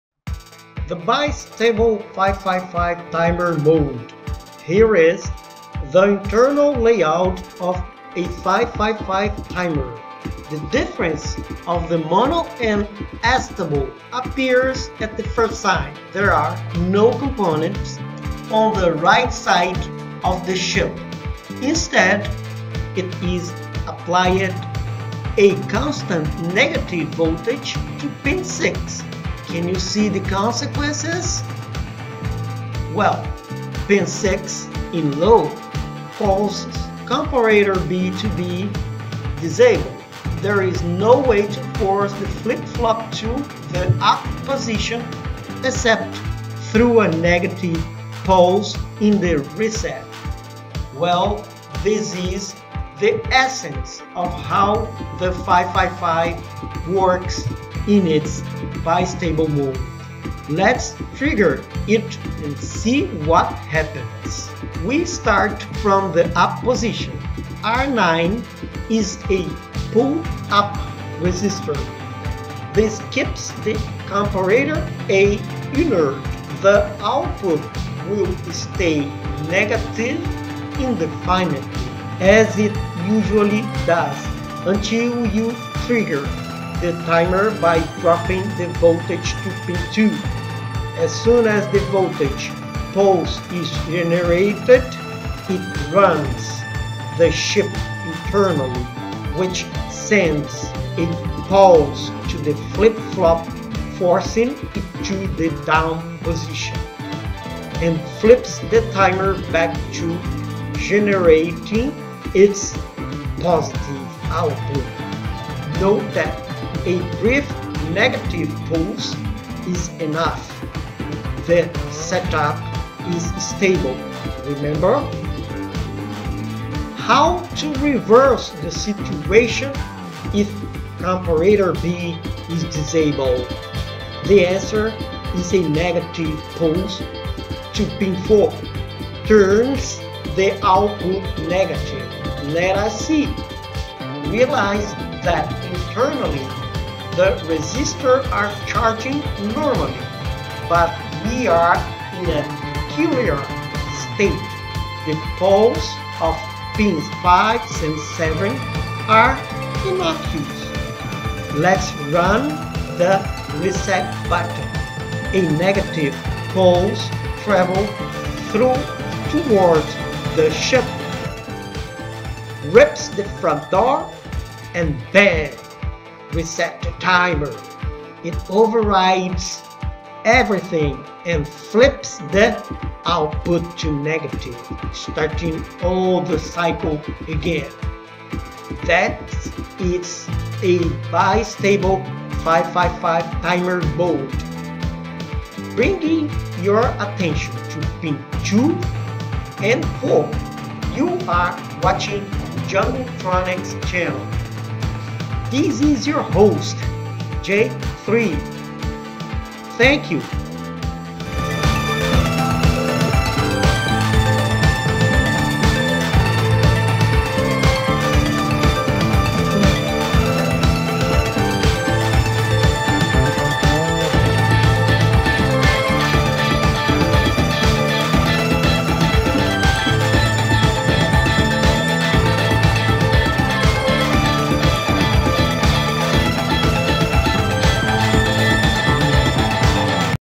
555 IC - Bistable Mode - Part II HowItWorks 05
Here is how it works of a 555 timer in its bistable configuration. jungletronics